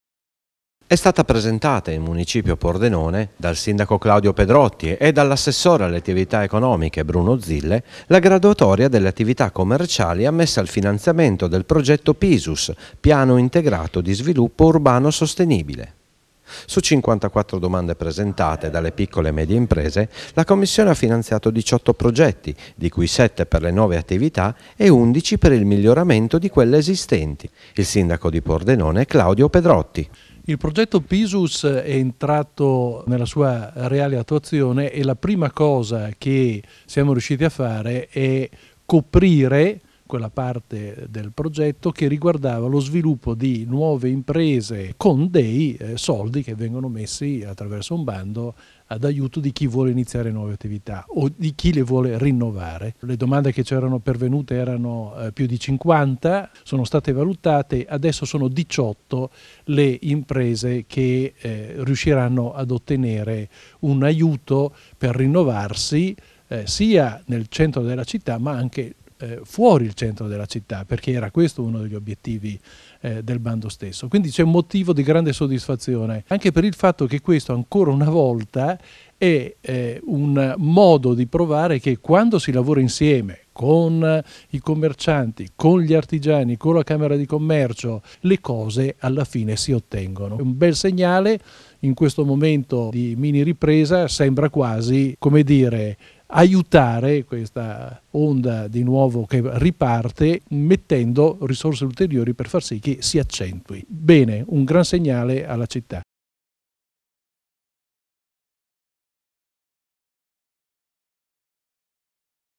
Pordenone: presentata la graduatoria dei finanziamenti Pisus alle PMI
È stata presentata in Municipio a Pordenone, dal  sindaco Claudio Pedrotti e dall’assessore  alle attività economiche Bruno Zille, la graduatoria delle attività commerciali ammesse al finanziamento del progetto PISUS, piano integrato di sviluppo urbano sostenibile.
Su 54 domande presentate dalle piccole e medie imprese, la Commissione  ha finanziato  18  progetti, di cui  7 per le nuove attività e 11 per il miglioramento di quelle esistenti.